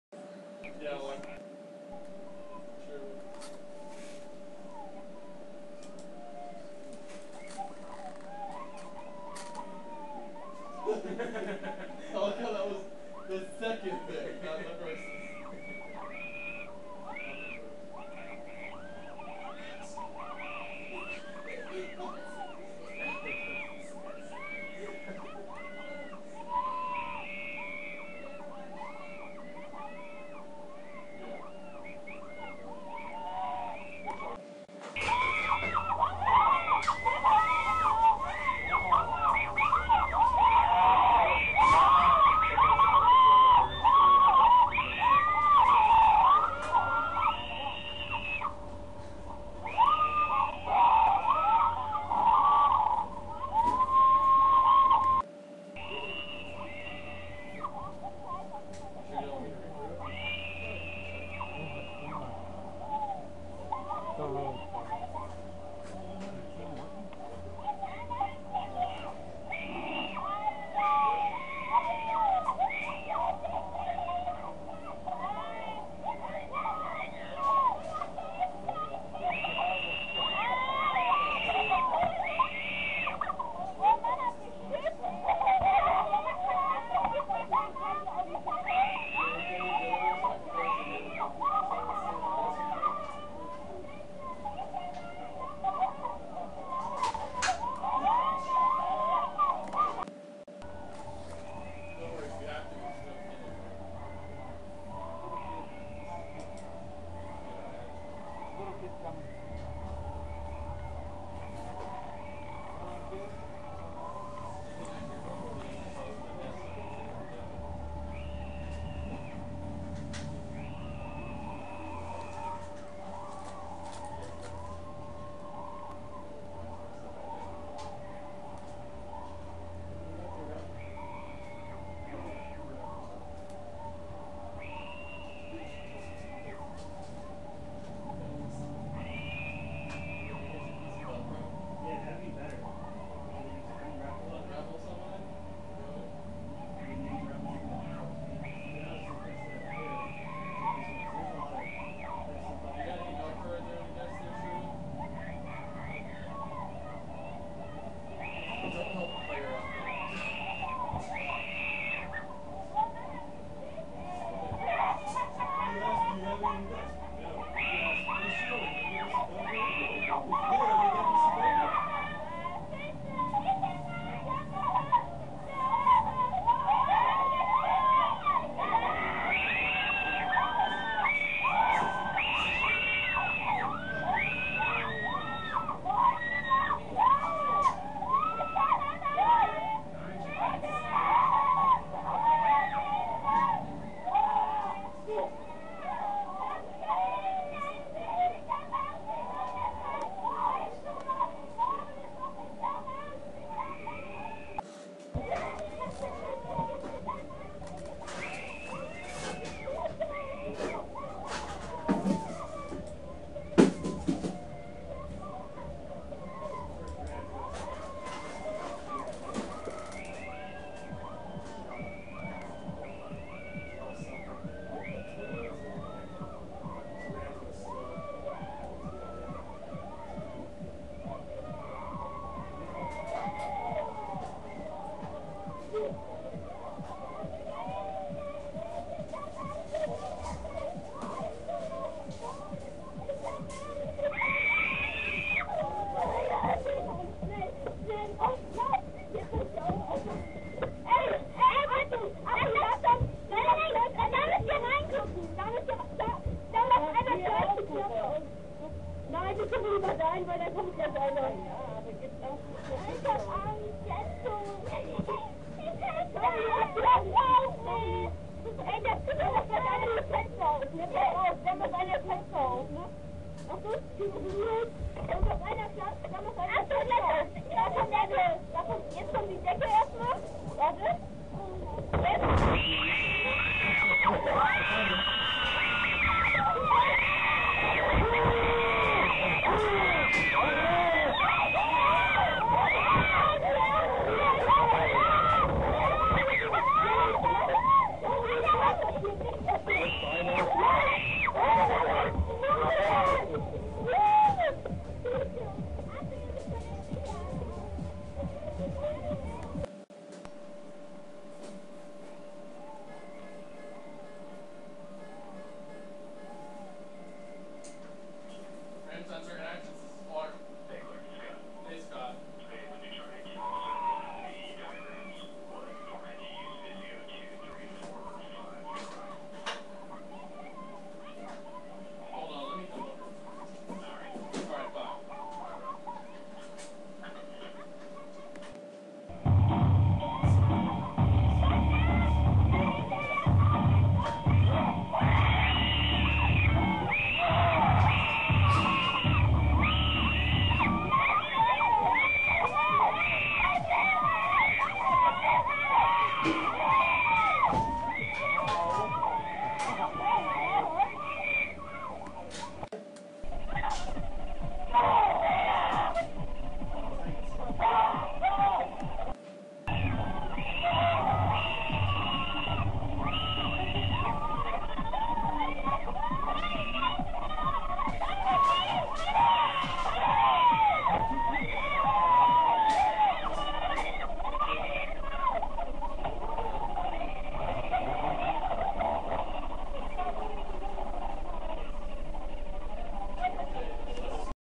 2012 Continous Screams
The haunt is very dark, nearly black.  Vampire room, Zombie Hallway, Torture Center, Crush Box, Unknown, Dots, and Clown Room.